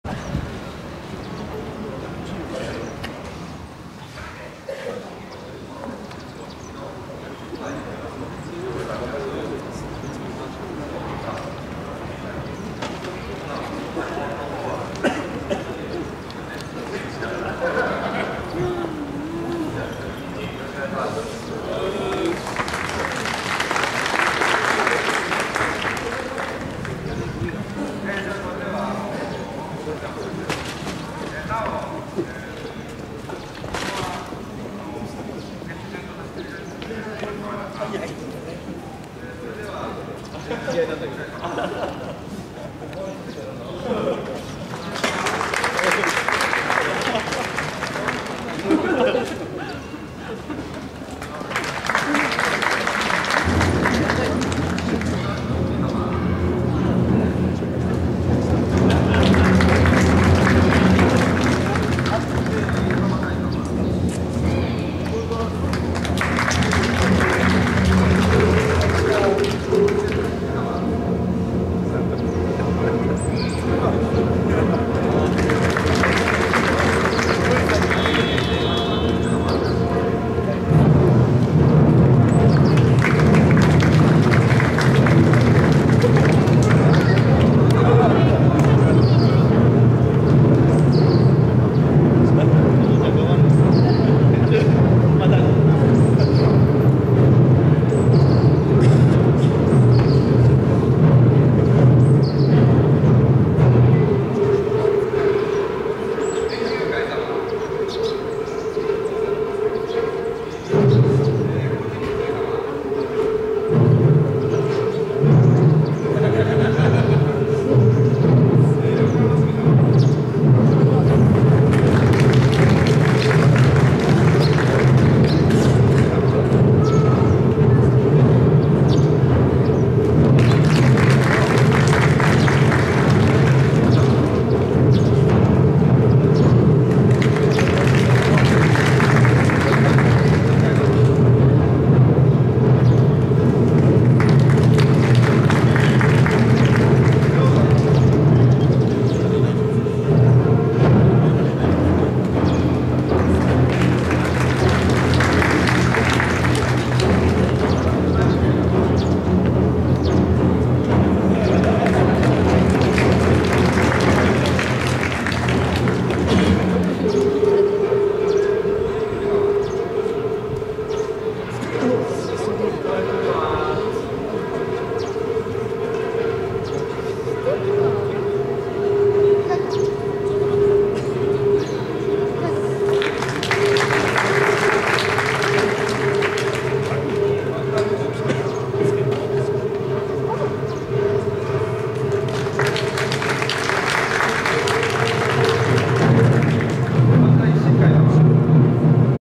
春の感謝祭り 出発神輿渡り御 相模大野南新町商店街2013年。
御神輿渡り御　皆　お疲れ様でした！！　太鼓も凄くpowerful でした　、神奈川県が最高です　２０１３年 東京　ジャパン　平成２５年の3月の24日です日曜日です　編集無し　其のまま RAW video （生）　Full AVCHD (HD)　by picua.